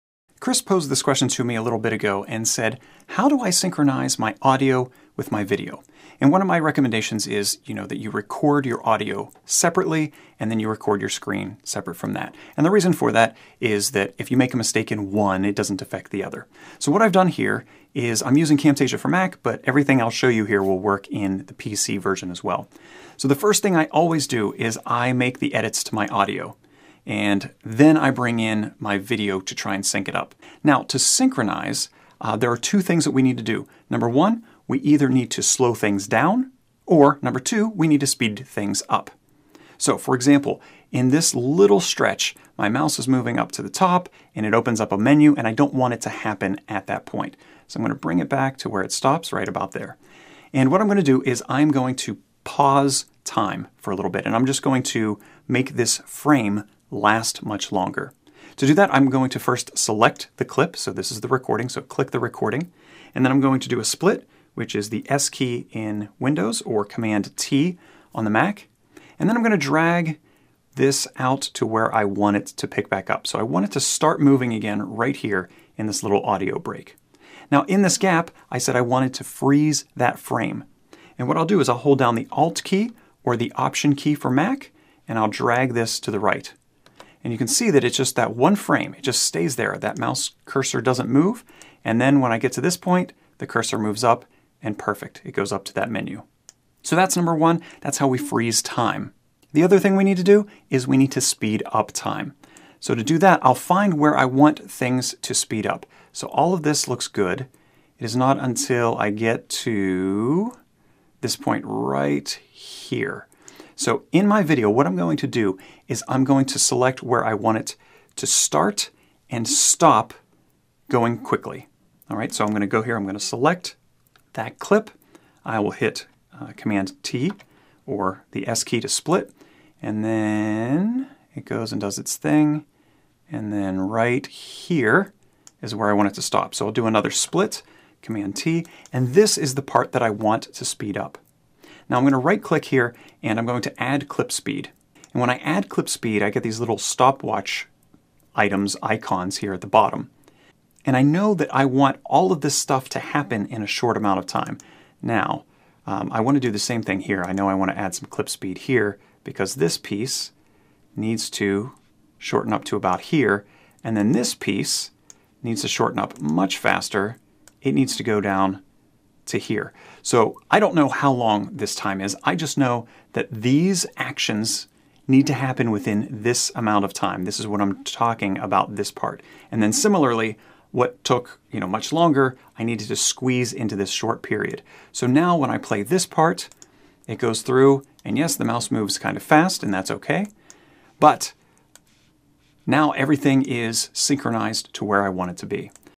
How to Sync Audio and Video in Camtasia
Speed things up or slow them down in Camtasia to sync your visuals with your audio.